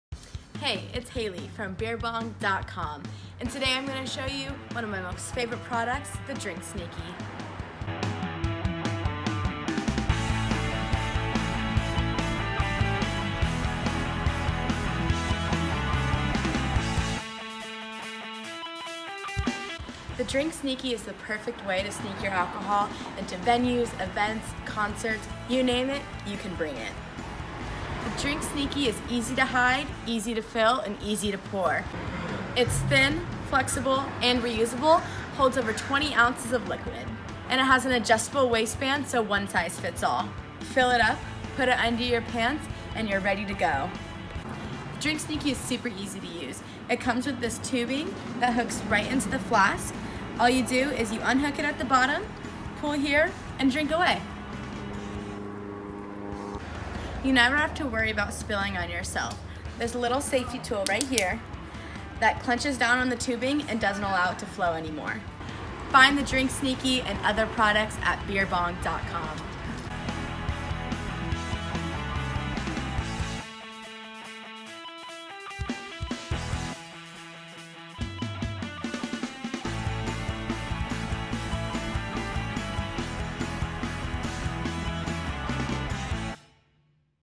Drink Sneaky
Hide your Liquor with Drink Sneaky. Take your liquor to games and concerts and no one will ever know.